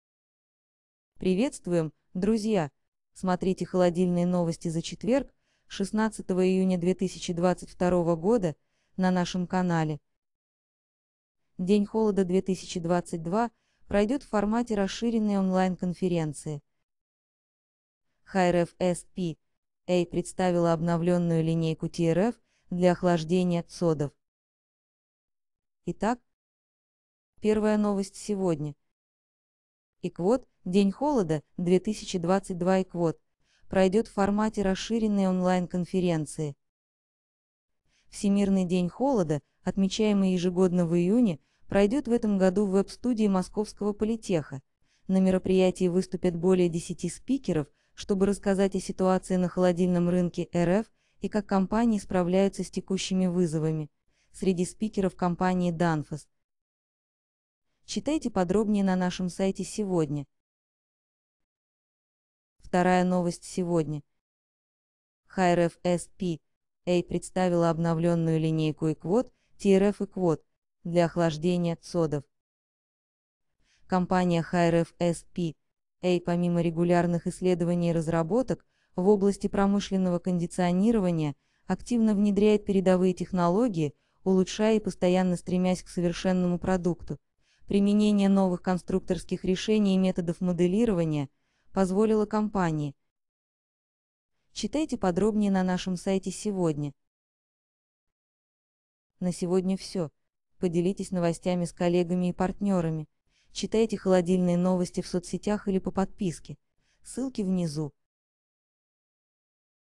"День холода 2022" пройдет онлайн / Обновленная линейка "TRF" для охлаждения ЦОДов от HiRef S.p.A.
Смотрите новости холодильной индустрии за четверг, 16 июня 2022 года.

Темы сегодняшнего выпуска новостей:
00:26 "День холода 2022" пройдет в формате расширенной онлайн-конференции.
01:01 HiRef S.p.A. представила обновленную линейку "TRF" для охлаждения ЦОДов.

Участвуйте в обсуждениях новостей, делитесь мнением и просите помощи у специалистов холодильной индустрии в группах: